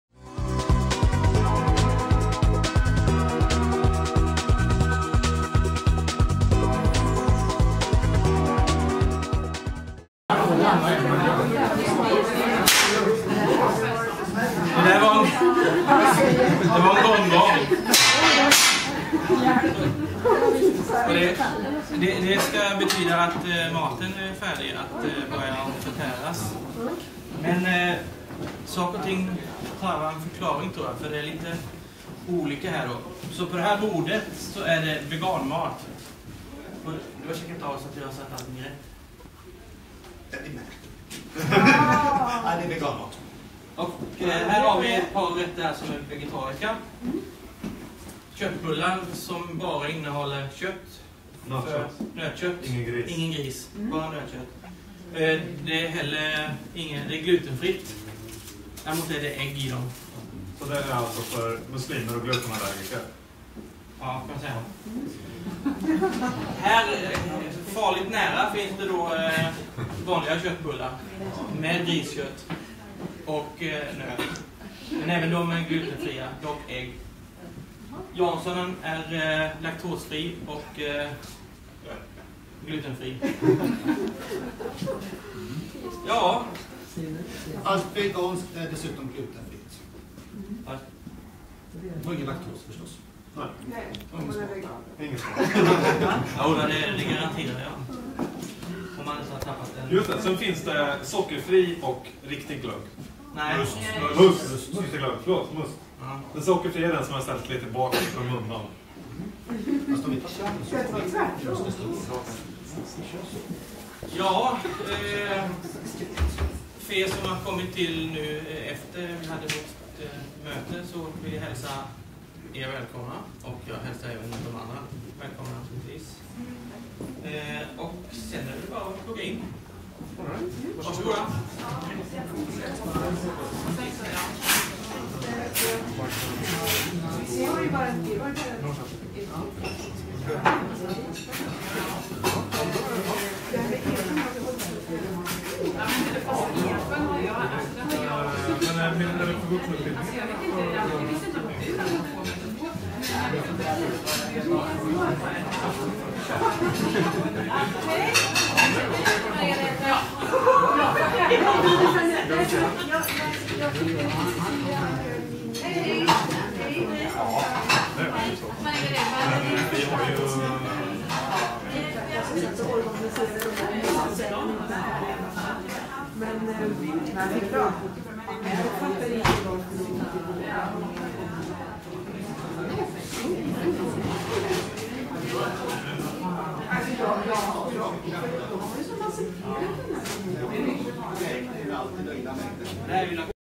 2008-12-14 - julfest
Mp-Norrköping har julfest 2008. Ordförande Peo Svensson, informationssekreterare Jens Lindell m.fl har ordnat ett rejält julbord för såväl veganer, vegetarianer som för dem som önskar husmanskost.